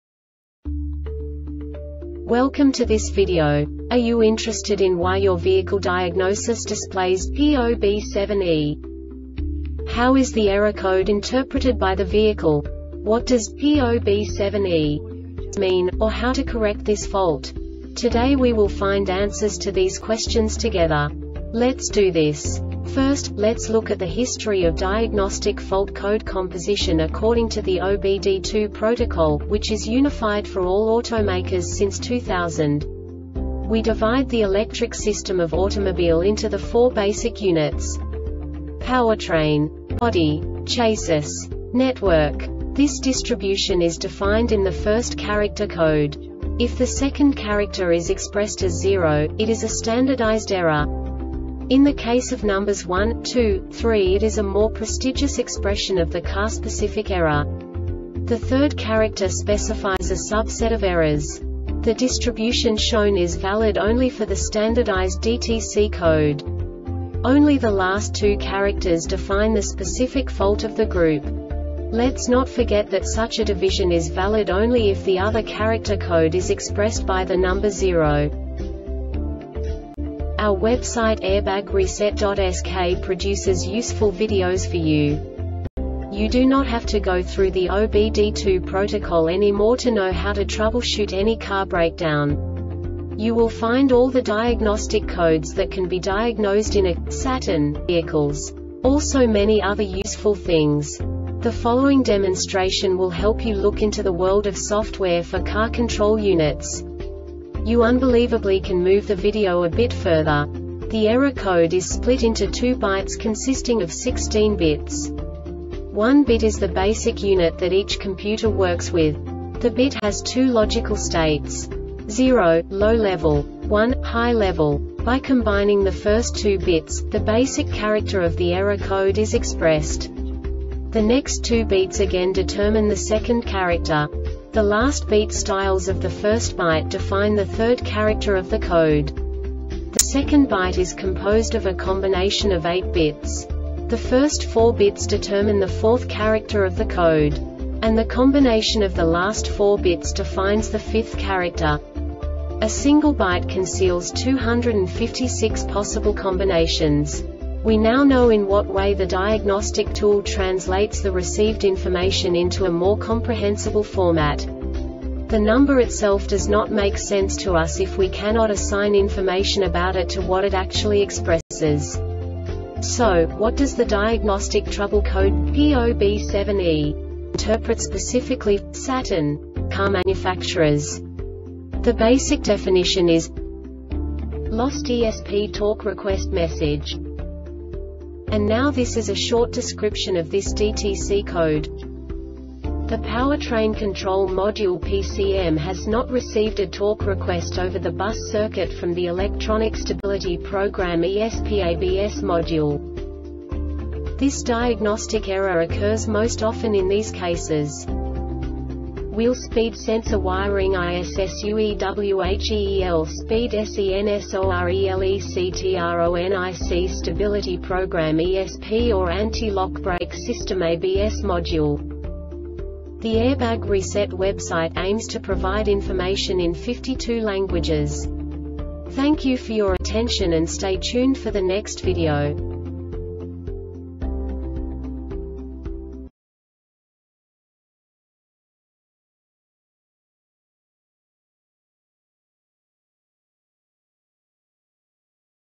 DTC Saturn P0B7E Short Explanation
Contents:
0:21 Basic DTC analysis according to OBD2 protocol standard.
1:48 Insight into programming
2:58 A diagnostically understandable description of the Diagnostic Trouble Code
3:05 Basic error definition

Make:
Saturn
Code:
P0B7E
Definition:
LOST ESP TORQUE REQUEST MESSAGE
Description:
The Powertrain Control Module (PCM) has not received a torque request over the bus circuit from the Electronic Stability Program (ESP) or ABS Module.
Cause:
WHEEL SPEED SENSOR WIRING ISSUEWHEEL SPEED SENSORELECTRONIC STABILITY PROGRAM (ESP) or ANTI-LOCK BRAKE SYSTEM (ABS) MODULE